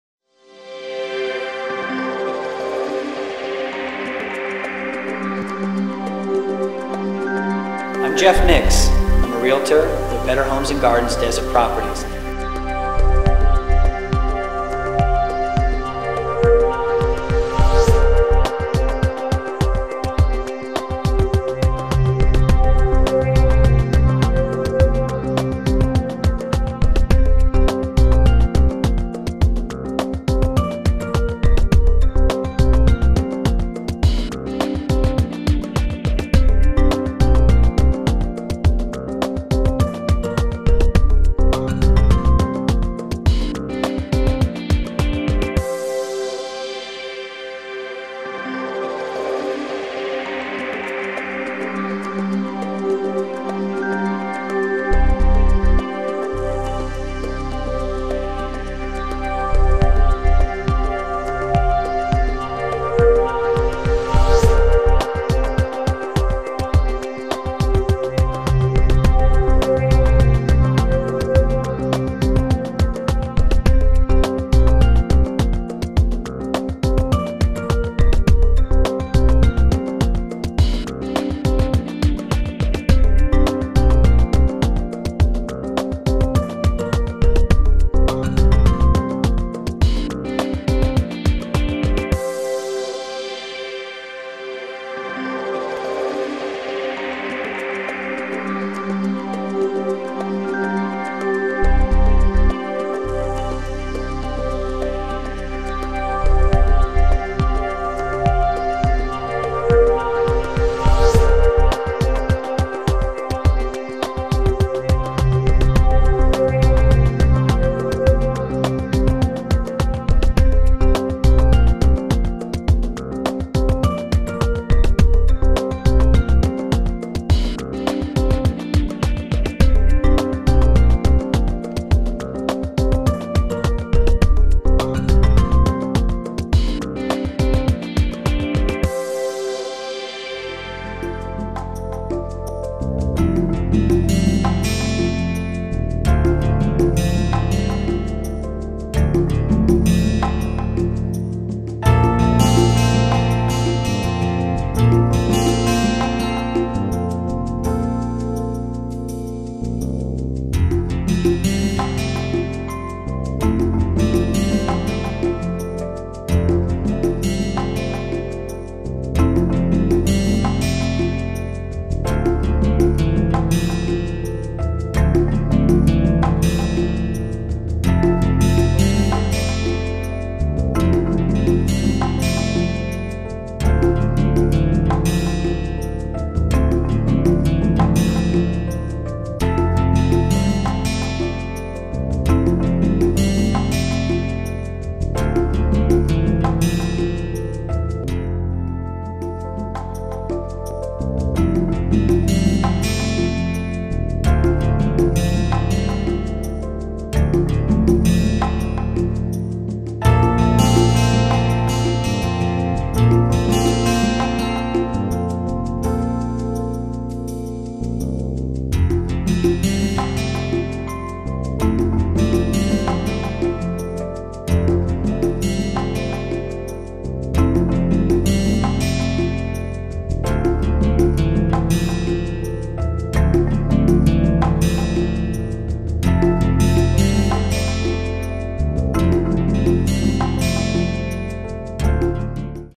Henderson Golf Course home | 241 Via Di Citta
Awesome golf course and mountain view home in the heart of Tuscany just lacated minutes from Lake Las Vegas.

More than 2400 sq ft, 4 bedrooms and 3 full baths.